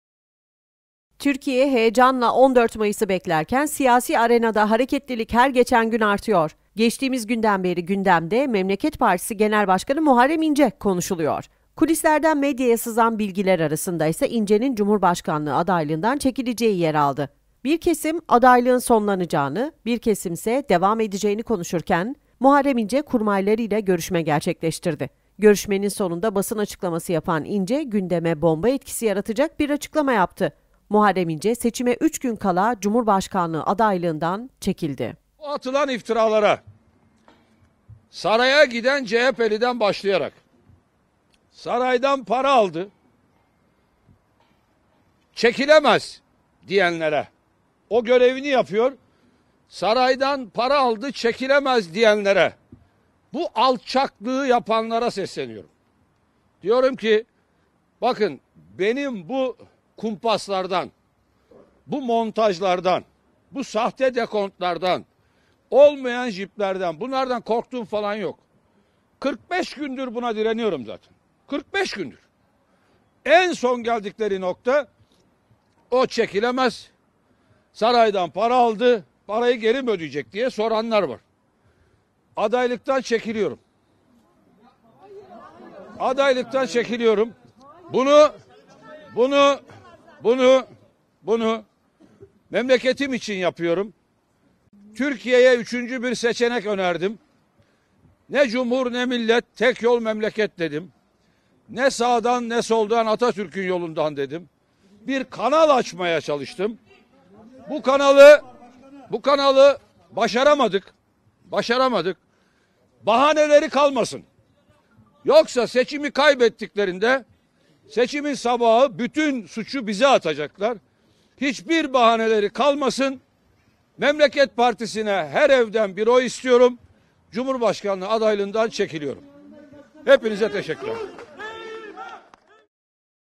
Muharrem İnce Cumhurbaşkanlığı Adaylığından Çekildi
Türkiye, heyecanla 14 Mayıs'ı beklerken, siyasi arenada hareketlilik her geçen gün artıyor.

Geçtiğimiz günden beri gündemde Memleket Partisi Genel Başkanı Muharrem İnce konuşuluyor.

Kulislerden medyaya sızan bilgiler arasında ise İnce'nin Cumhurbaşkanlığı adaylığından çekileceği yer aldı.

Bir kesim adaylığın sonlanacağını, bir kesim ise devam edeceğini konuşurken Muharrem İnce, kurmayları ile görüşme gerçekleştirdi.

Görüşmenin sonunda basın açıklaması yapan İnce, gündeme bomba etkisi yaratacak bir açıklama yaptı.

Muharrem İnce, seçime 3 gün kala cumhurbaşkanlığı adaylığından çekildi.